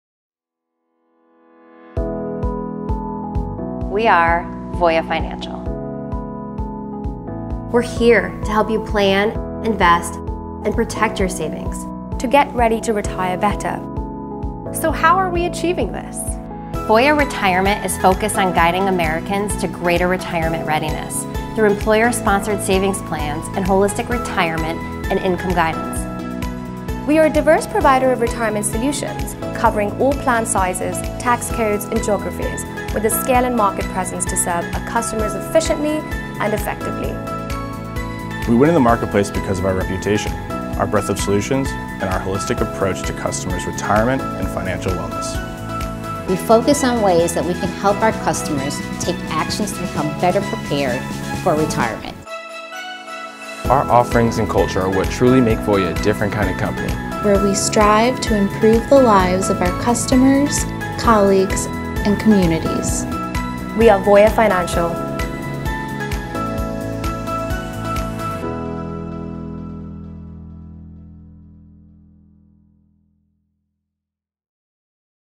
Voya Overview Video – Retirement
Voya Retirement is focused on guiding Americans to greater retirement readiness through employer-sponsored savings plans and holistic retirement and income guidance.